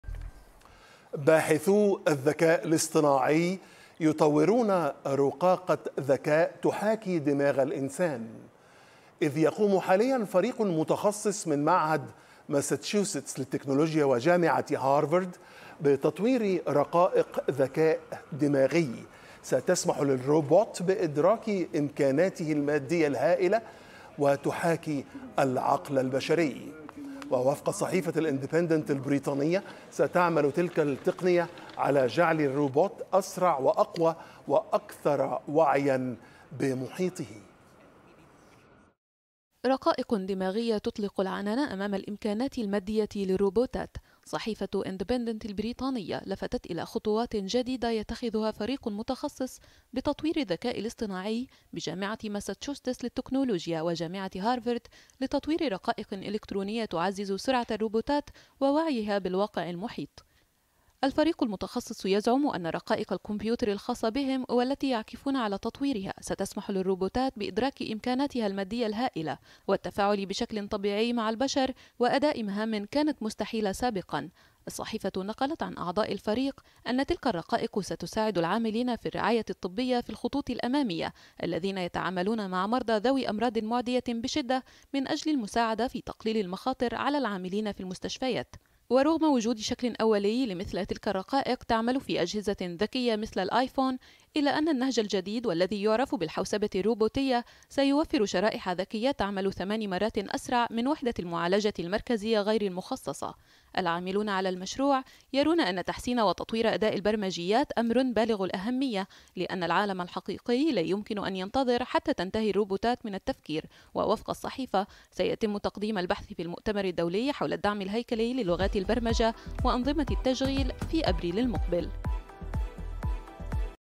باحثون يبتكرون "أدمغة" روبوتات تمكّنها من أداء مهام غير مسبوقة - أخبار الشرق
ابتكر باحثون في معهد ماساتشوستس للتكنولوجيا "إم آي تي"، شرائح متطورة لتعويض وحدة المعالجة المركزية التي تشكل"عقل" الروبوتات، تجعلها أسرع وأكثر إداركاً بمحيطها، ما يؤهلها للقيام بمهام بشرية غير مسبوقة.